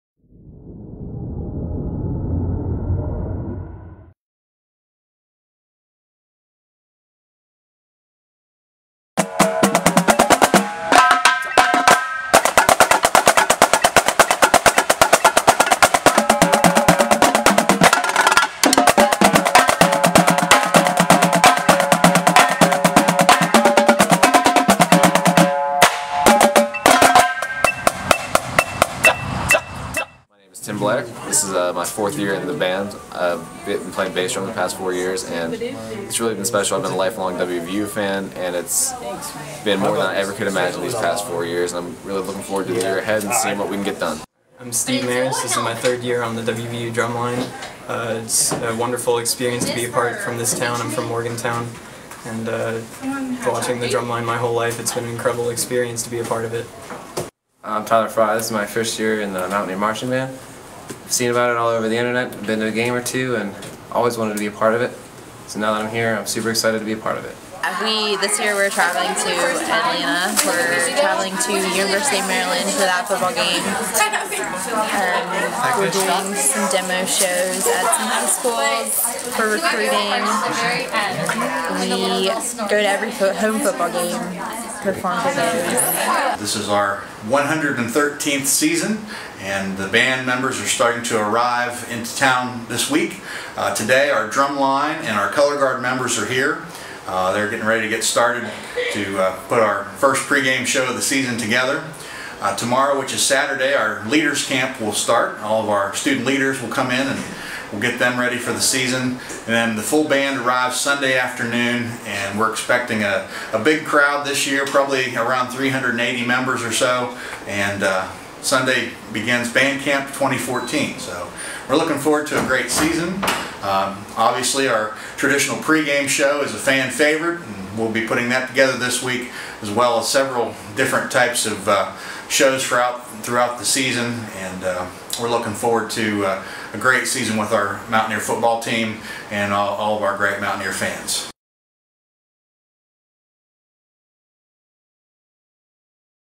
WVU drum line, color guard begin practice 8-8-14
The WVU drum line and color guard began practice Friday.